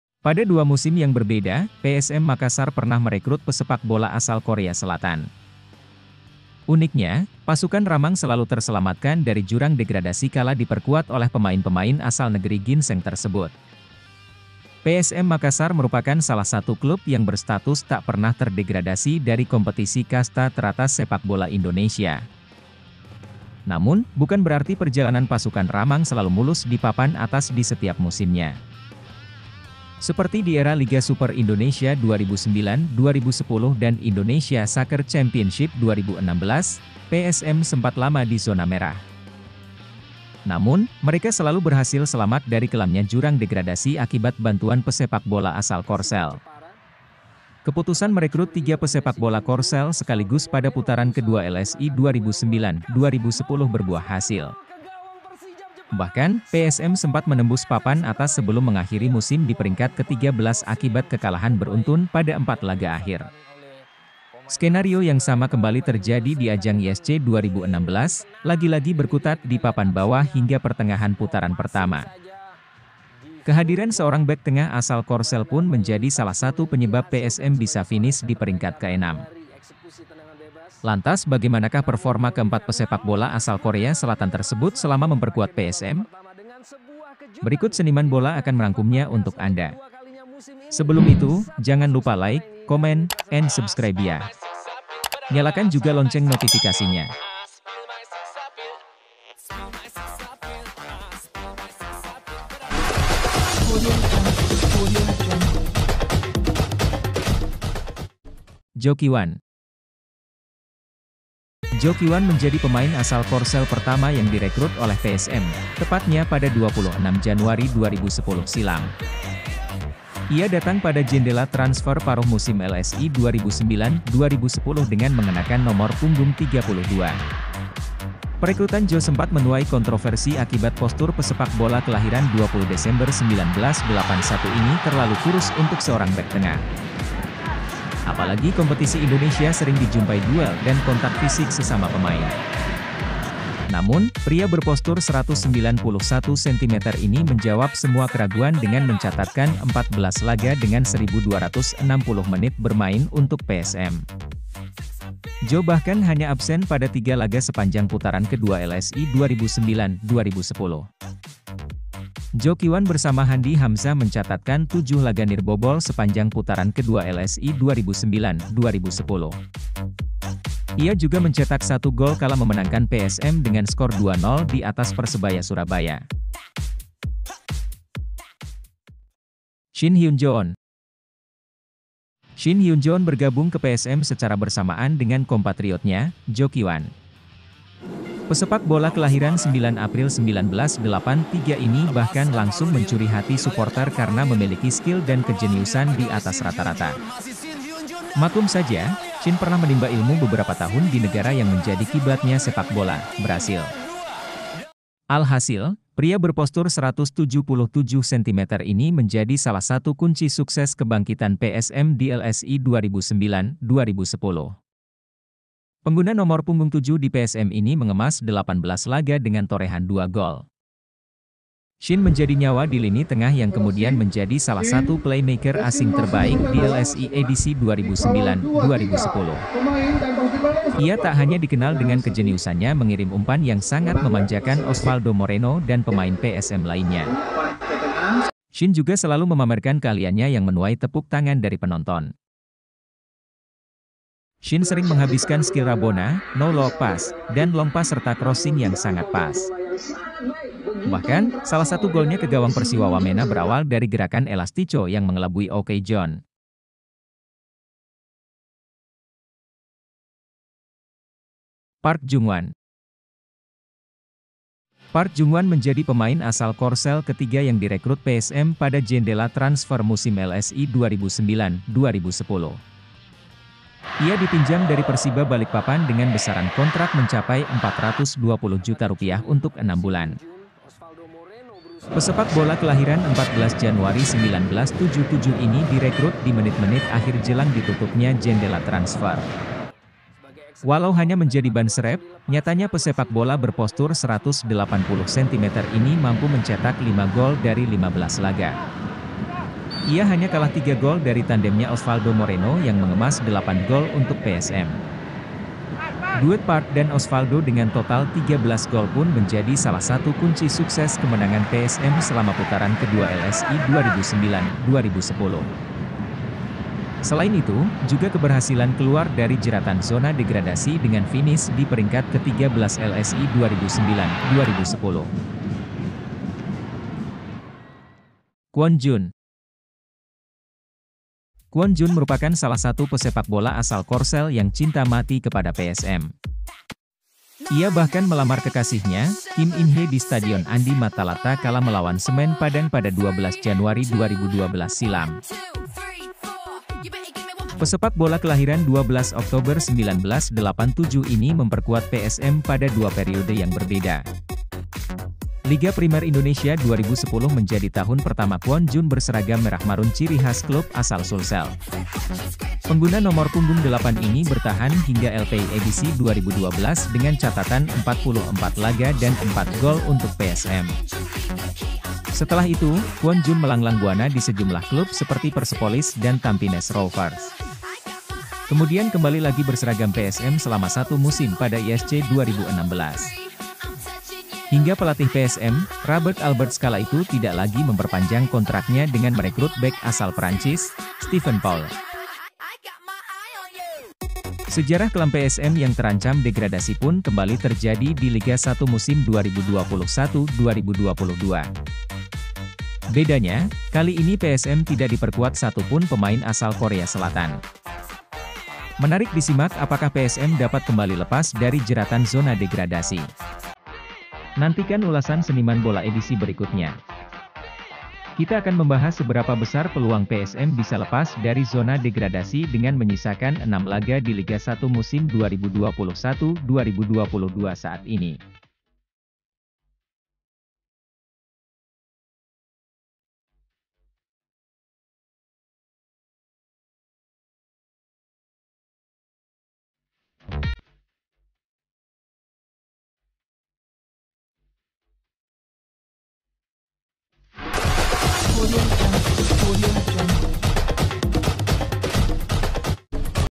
Pemain Korea Selatan yang Selamatkan PSM Makassar dari Degradasi
Pada dua musim yang berbeda, PSM Makassar pernah merekrut pesepak bola asal Korea Selatan.

Uniknya, Pasukan Ramang selalu terselamatkan dari jurang degradasi kala diperkuat oleh pemain-pemain asal Negeri Ginseng tersebut.

PSM Makassar merupakan salah satu klub yang berstatus tak pernah terdegradasi dari kompetisi kasta teratas sepakbola Indonesia.

Namun, bukan berarti perjalanan Pasukan Ramang selalu mulus di papan atas di setiap musimnya.

Seperti di era Liga Super Indonesia (LSI) 2009/10 dan Indonesia Soccer Championship (ISC) 2016, PSM sempat lama di zona merah.

Namun, mereka selalu berhasil selamat dari kelamnya jurang degradasi akibat bantuan pesepakbola asal Korsel.

Keputusan merekrut tiga pesepakbola Korsel sekaligus pada putaran kedua LSI 2009/10 berbuah hasil.

Bahkan, PSM sempat menembus papan atas sebelum mengakhiri musim diperingkat ke-13 akibat kekalahan beruntun pada empat laga akhir.

Skenario yang sama kembali terjadi diajang ISC 2016, lagi-lagi berkutat di papan bawah hingga pertengahan putaran pertama.

Kehadiran seorang bek tengah asal Korsel pun menjadi salah satu penyebab PSM bisa finish diperingkat keenam.

Lantas bagaimanakah performa keempat pesepakbola asal Korea Selatan tersebut selama memperkuat PSM Makassar?

Berikut Seniman Bola akan merangkumnya untuk anda :Pemain Korsel yang Dua Kali Selamatkan PSM dari Degradasi

Berikut Seniman Bola akan merangkumnya untuk anda :

psm makassar
pemain korea
pemain korsel
joo ki-hwan
shin hyun joon
park jung-hwan
kwon jun